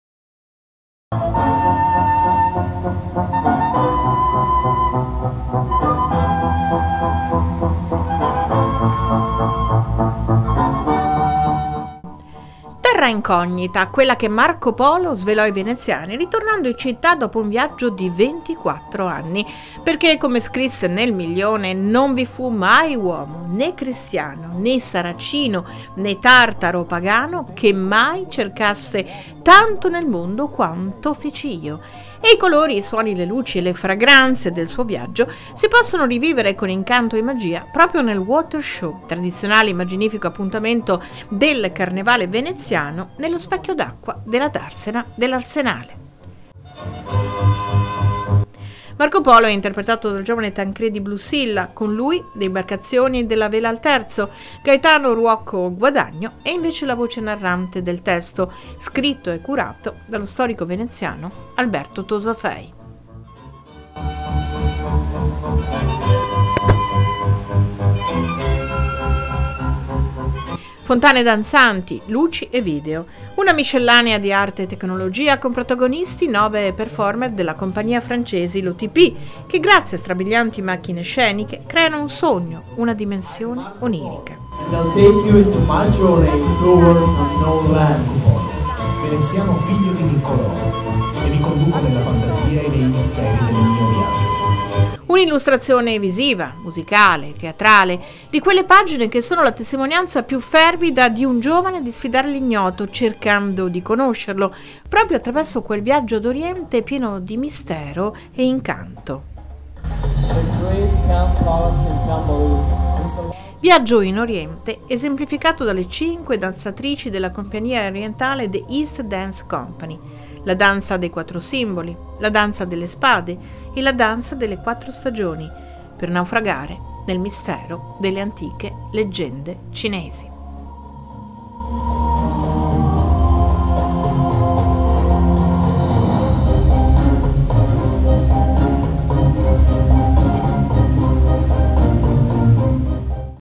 LA FESTA SULL'ACQUA
Magia ed incanto all'Arsenale di Venezia. In bacino la festa sull'acqua dedicata alla Terra incognita di Marco Polo, tema del Carnevale di quest'anno